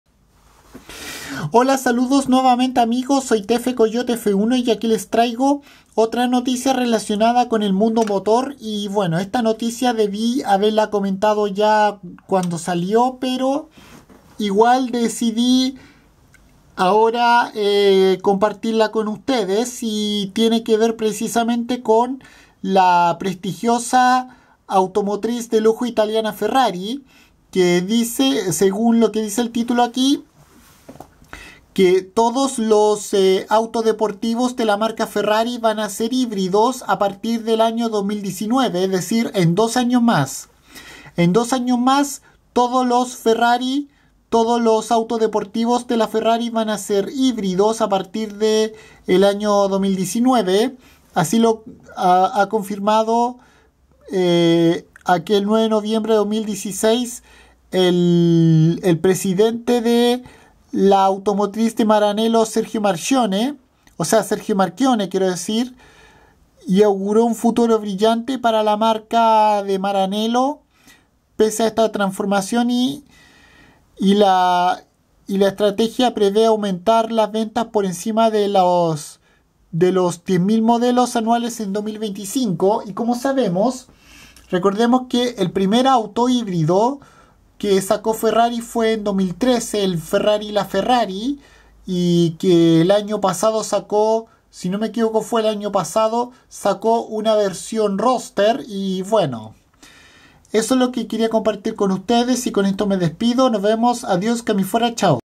Todos los Ferrari serán híbridos a partir del año 2019
A partir del año 2019 todos los autos deportivos de la automotriz italiana de Maranello Ferrari serán híbridos según lo confirmó hace algunos meses el presidente de la prestigiosa marca de lujo Sergio Marchionne